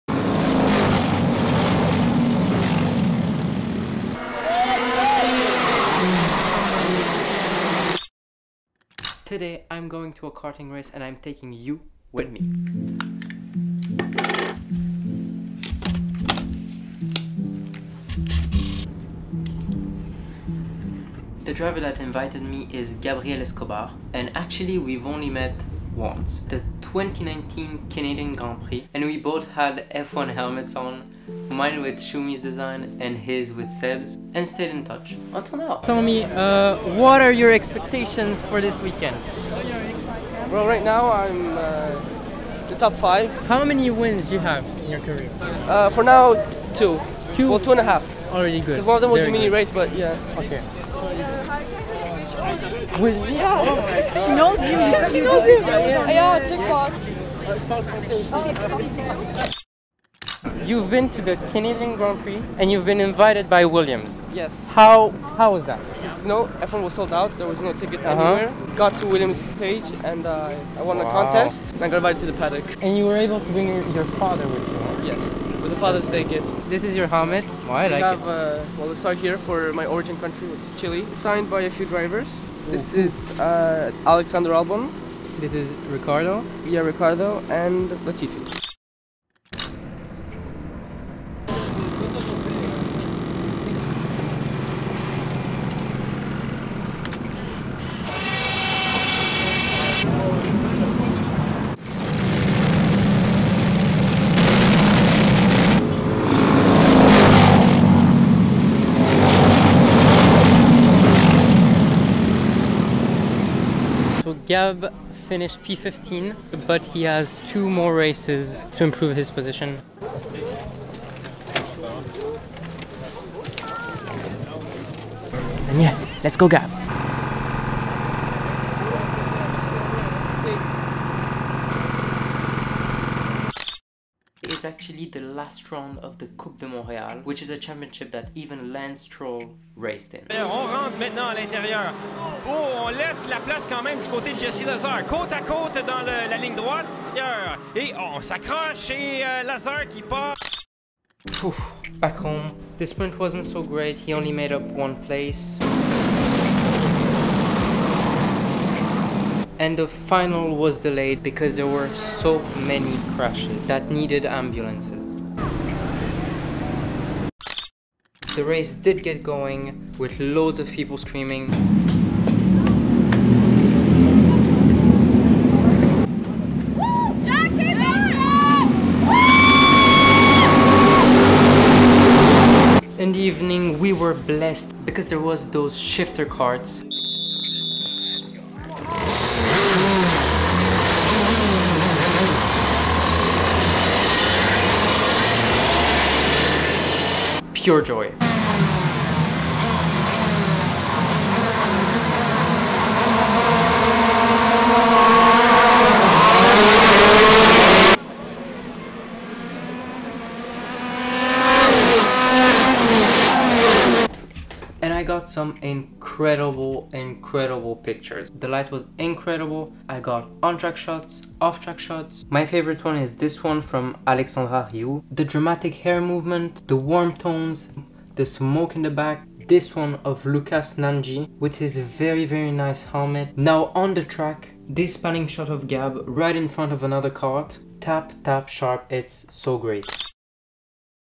KARTING RACE DAY VLOG - SH karting
It was an absolute pleasure to spend the day with Gab at SH karting for the final round of the coupe de Montreal! With fast karts, burning rubber and the smell of fuel... PURE PLEASURE! An incredible adventure, hoping to have more opportunities like these in the future!